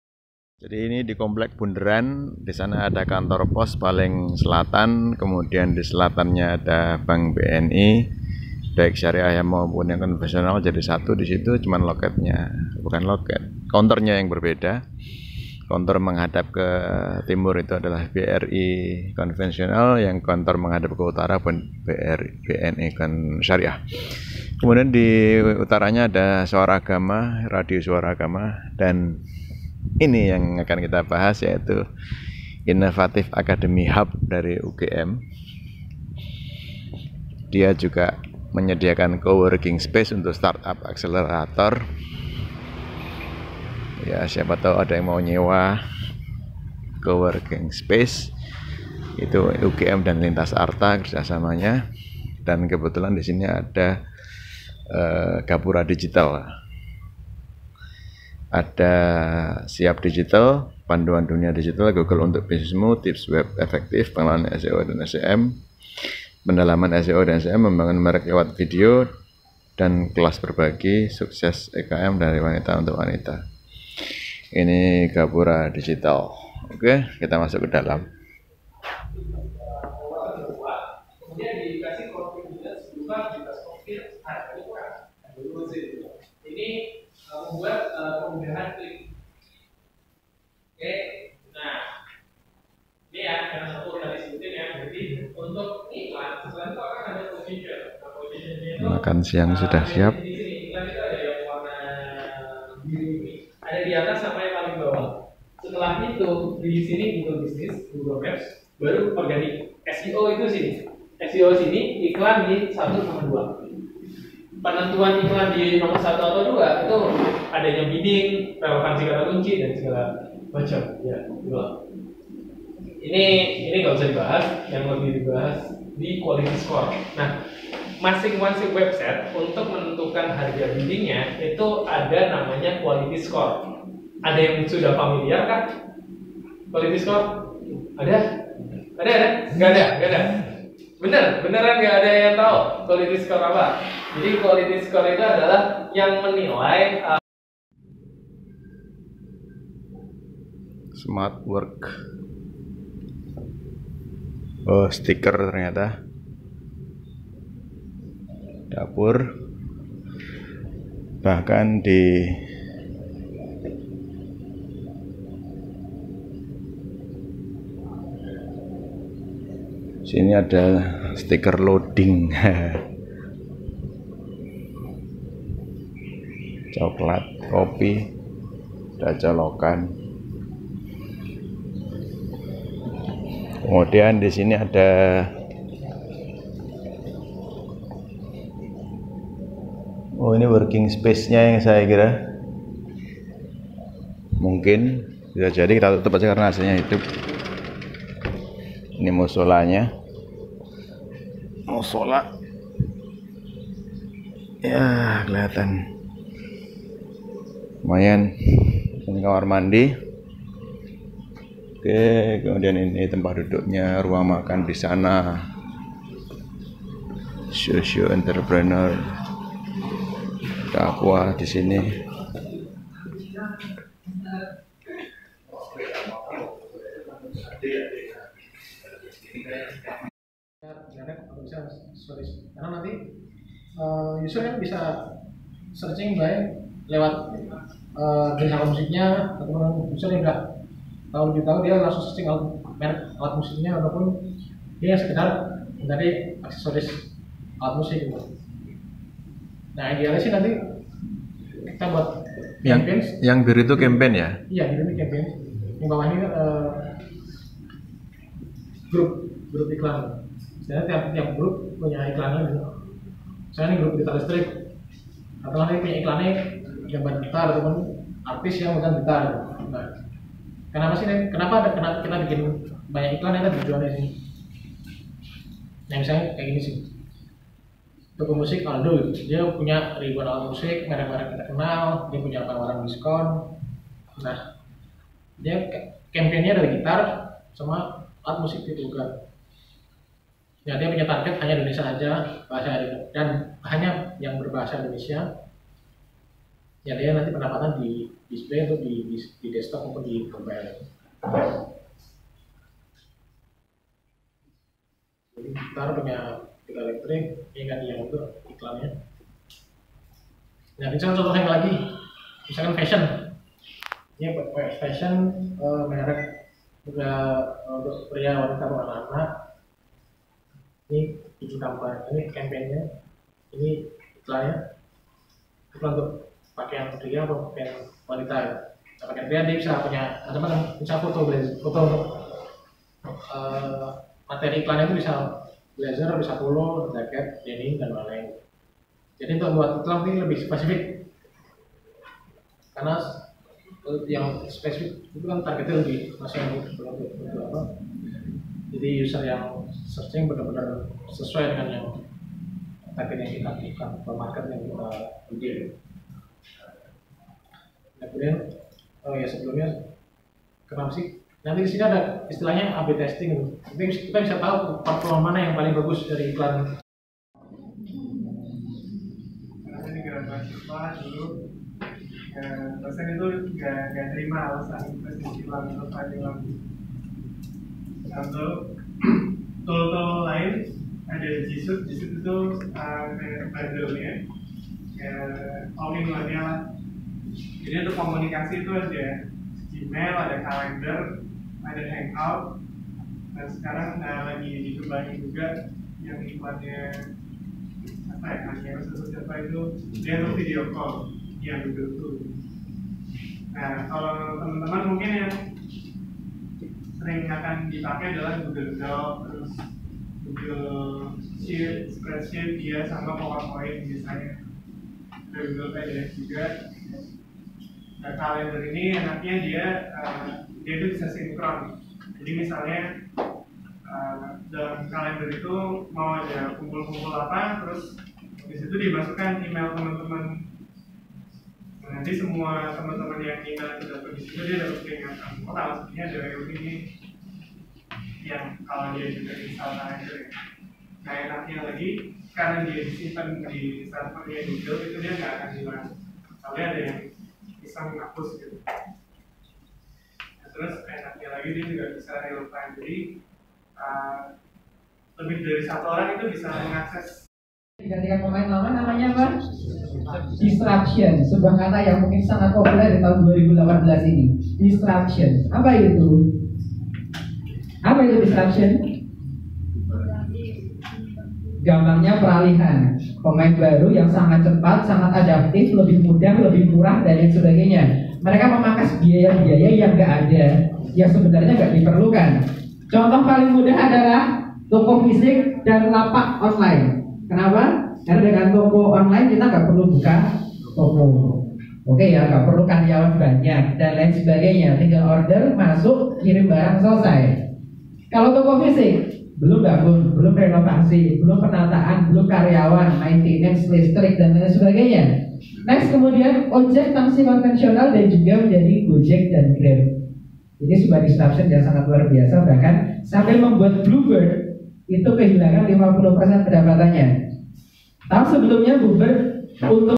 Innovative Academy Hub
Innovative Academy Hub, co-working space yang dibangun dengan mengedepankan nilai-nilai kearifan lokal, SESRAWUNGAN, REMBUGAN dan INSPIRASI yang dalam bahasa Indonesia berarti semangat gotong-royong, kebersamaan, keberagaman, inovasi dan kreativitas. Co-working space yang dibangun melalui sinergi Universitas Gadjah Mada dan Lintasarta ini tidak hanya diperuntukkan bagi warga UGM, tapi juga bagi masyarakat luas yang ingin mengembangkan aplikasi digital. Keberadaan coworking space ini diharapkan dapat semakin memperkuat ekosistem inovasi dan hilirisasi di lingkungan kampus. Pada tahap awal 11 startup telah berkomitmen untuk berproses di tahap akselerasi melalui Innovative Academy Hub, yaitu Wemary, Galanggo, Pasienia, Pijar, Iwak, Muncak, Villageria, Bantu Ternak, Adsiconic, Majapahit, Calty Farm. Co-working space menyediakan ruang kerja yang nyaman, bergaya anak muda, dan dilengkapi dengan high speed internet, clouds, serta mentor-mentor dari para pakar aplikasi dan bisnis digital.